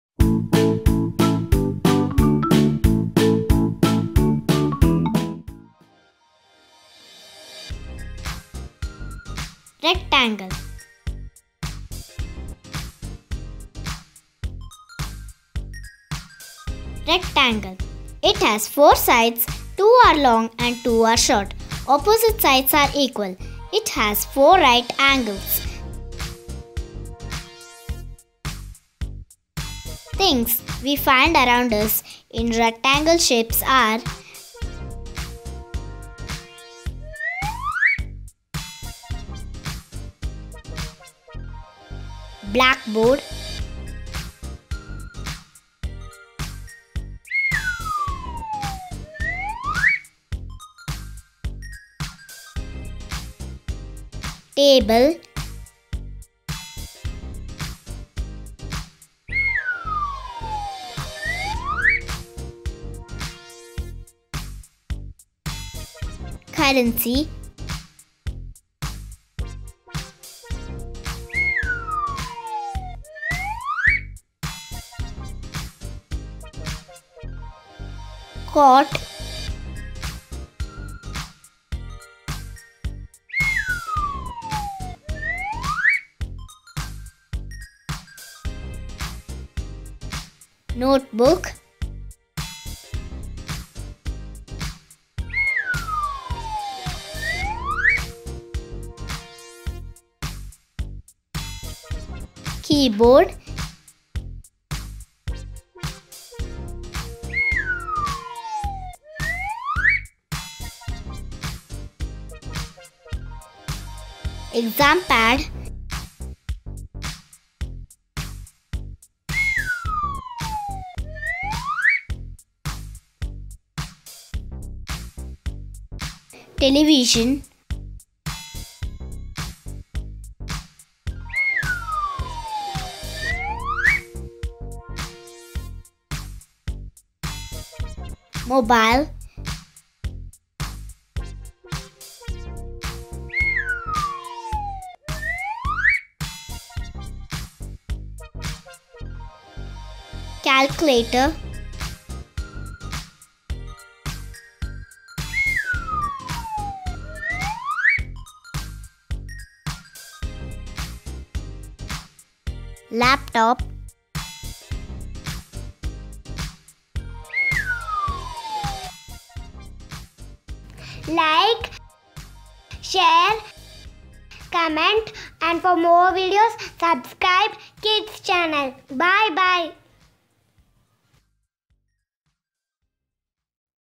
Learn shapes - Rectangle with pictures and sounds | Kids Channel
Learning about shapes is fun; it improves the kids ability to communicate with the things.
A shape plays an important role to improve their learning skills, while playing with toys, eating any eatables, fruits, veggies, visiting any places, travelling.
kids can recognize the pictures or things or an surfaces of the area, and are able to know more about the shapes.
For more names of shape …. Keep watching kids channel.

To learn more about activities join our kids channel group.